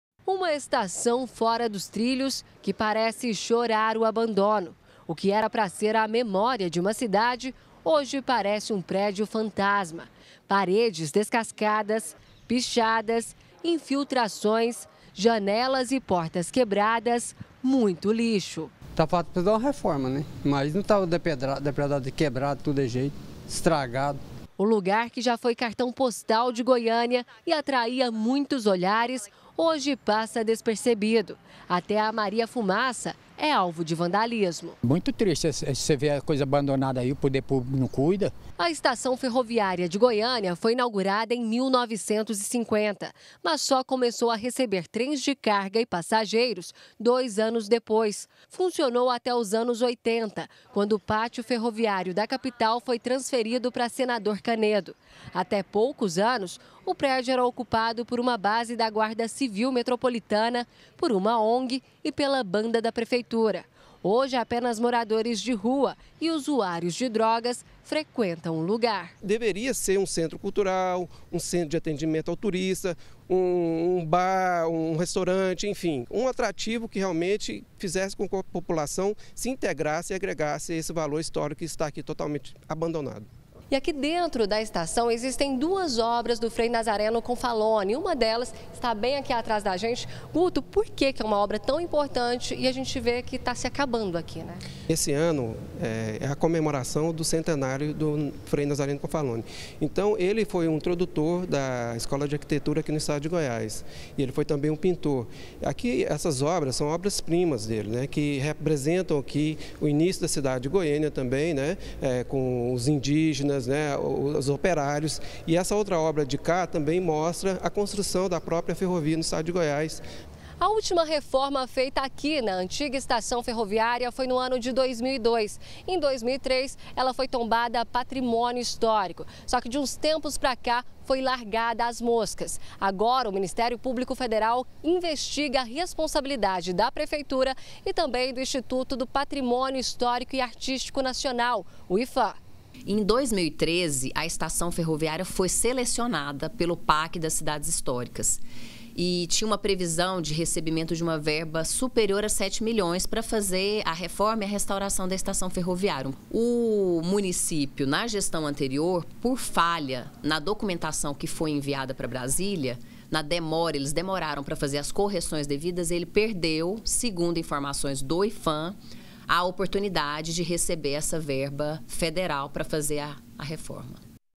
GR - Ministério Público investiga: Abandono da estação ferroviária - 06-12-2017
O Ministério Público Federal investiga a omissão da prefeitura de Goiânia em relação à reforma e conservação da estação ferroviária, que fica perto da rodoviária. A prefeitura teria ignorado um processo do governo federal para receber sete milhões de reais para a restauração da estação.